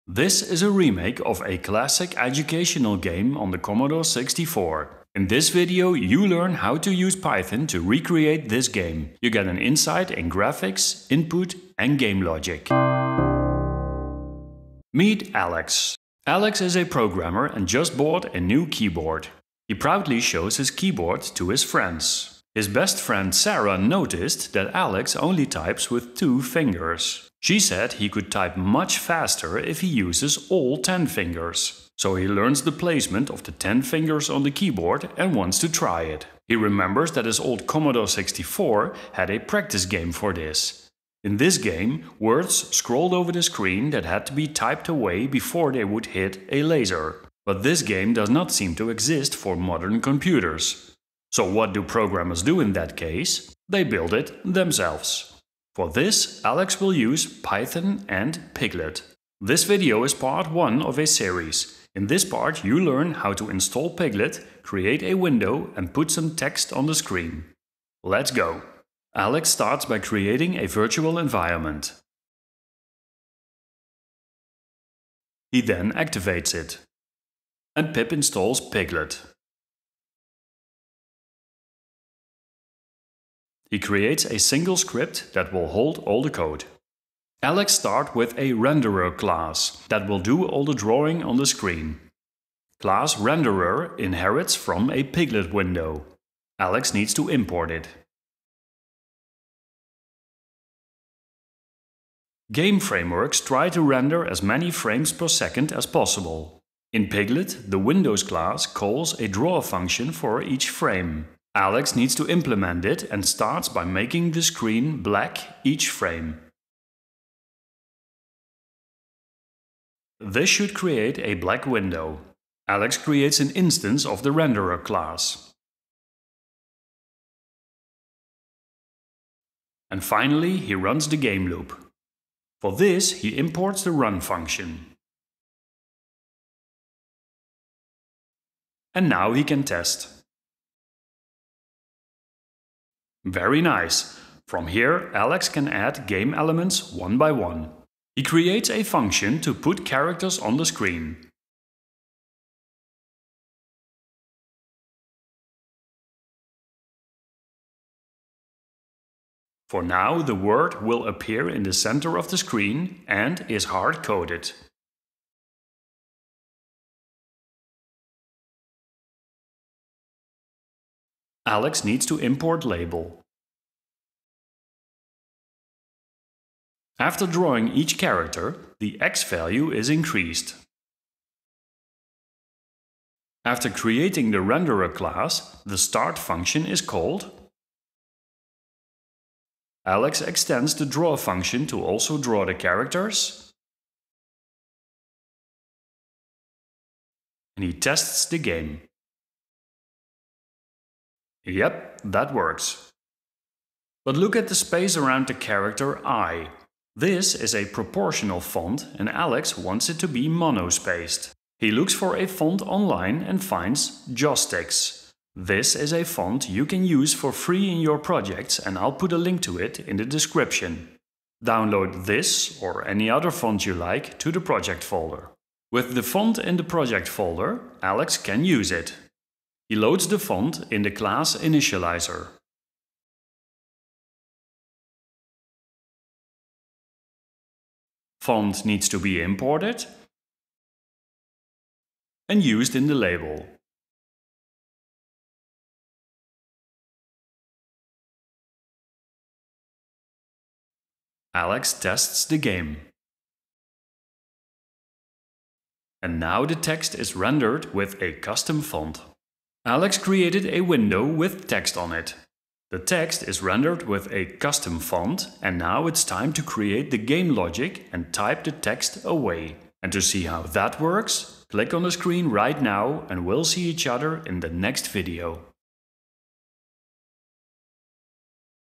1 Pyglet window and text - Recreate classic C64 game in Python with Pyglet
Re-create a classic educational Commodore 64 game with Python and Pyglet. Learn how to create a window, put text on the screen, receive keyboard input, separate your logic from rendering and how to create beautiful animations!

0:18 What are you going to build?
0:40 Tempo Typing on the C64
1:08 Create a virtual environment and install Pyglet
1:27 Create a Pyglet window
2:27 Text on the screen
3:30 Custom font

🎓Like my style of teaching? Try free chapters of online courses

L I N K S   I N   T H E   V I D E O